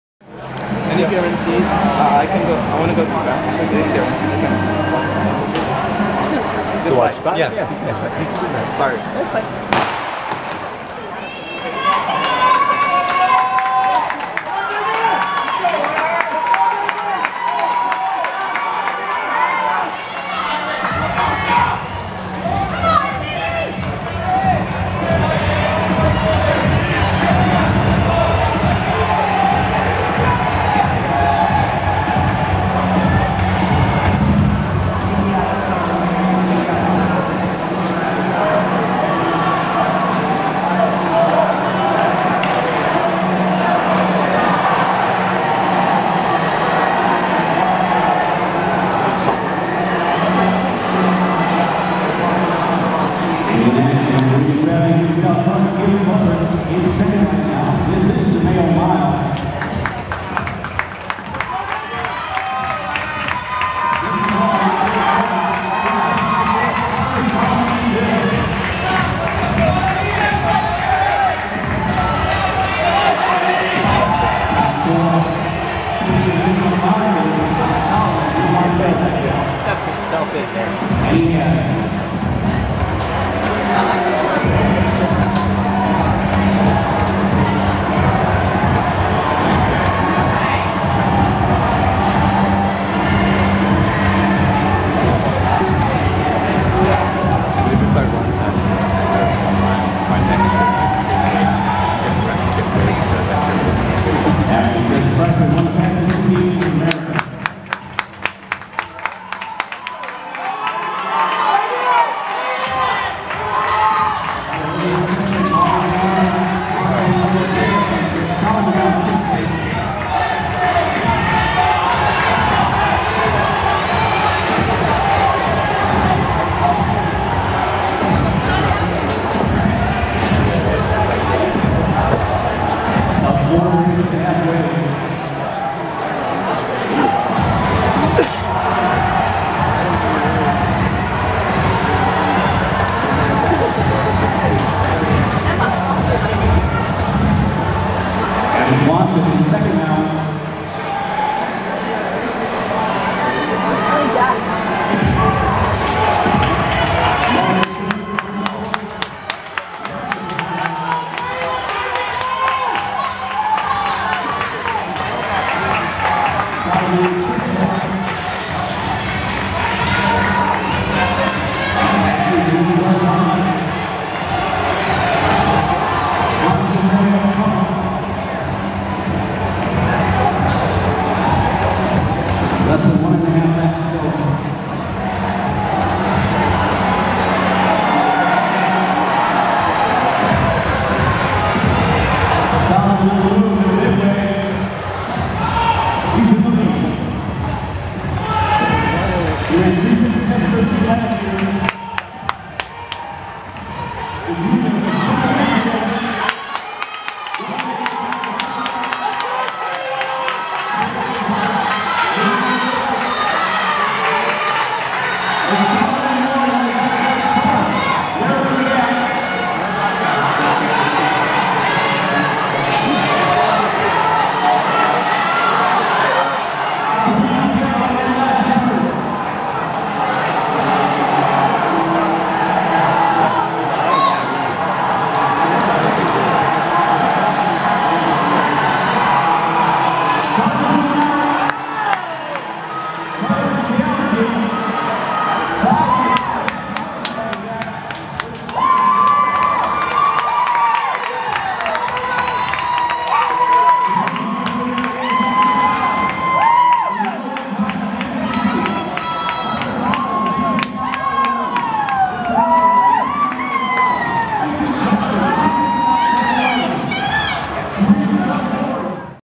Men's Championship Mile - 2024 Meyo Invitational
Meyo Invitational
South Bend, Indiana
February 3, 2024
(320m unbanked track)
Men's Championship Mile Results:
     1) Caleb Futter (Grand Valley) - 3:58.65
     2) Carter Solomon (Notre Dame) - 3:59.01
     3) Vincent Mauri (Notre Dame) - 3:59.05
     4) Ethan Coleman (Notre Dame) - 3:59.31
     5) William Merrick (Illinois) - 4:01.26
     6) Rishabh Prakash (Notre Dame) - 4:02.24
     7) Damian Rodriguez (DePaul) - 4:03.24
     8) Ethan Yost (Kent State) - 4:03.25
     9) Adam Swanson (Eastern Illinois) - 4:03.59
   10) Jayde Rosslee (Bradley) - 4:08.31
   11) Will O'Brien (Illinois) - 4:22.68
          Daelen Ackley (Notre Dame) - DNF